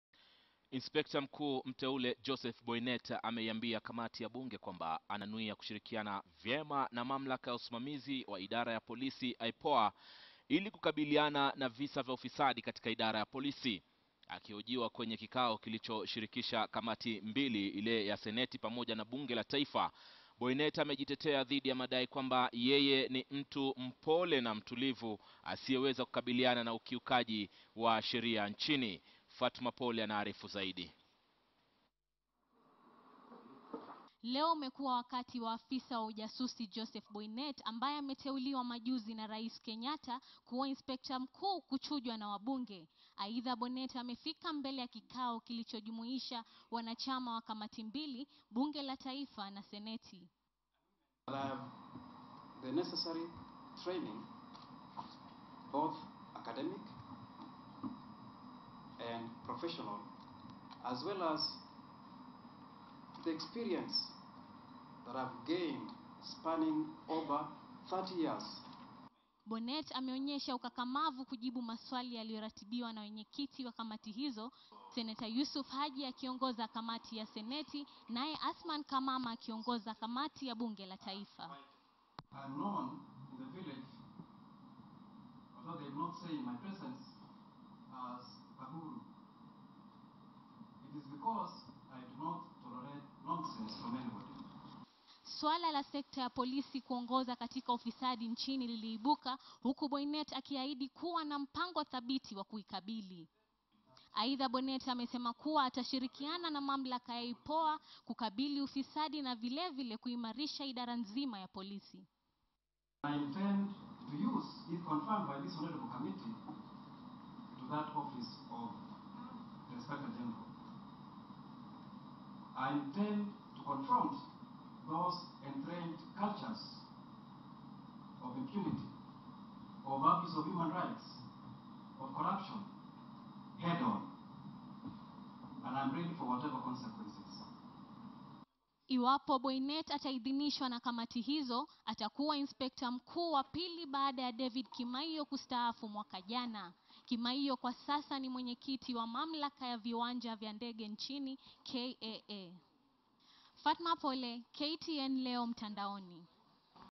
Inspekta mkuu mteule Joseph Boinnet aahidi kushirikiana na IPOA kumaliza ufisadi idara ya polisi
Inspekta mkuu mteule Joseph Boinnet ameiambia kamati ya bunge kwamba ananuia kushirikiana vyema na mamlaka ya usimamzi wa idara ya polisi IPOA ili kukabili visa vya ufisadi katika idara ya polisi. Akihojiwa kwenye kikao kilichoshirikisha kamati mbili; za seneti na bunge la taifa, Boinet amejitetea dhidi ya madai kwamba yeye ni mtu mpole asiyeweza kukabili ukiukaji sheria nchini.

Courtesy of KTN